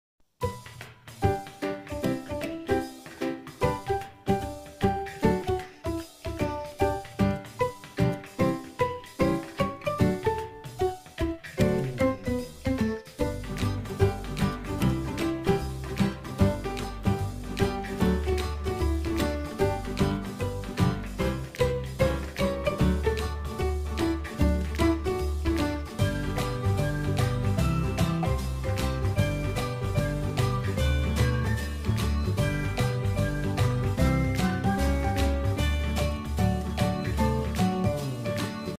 Come back, my sweet mouse! 🐭
Siamese kitten playing with her mouse toy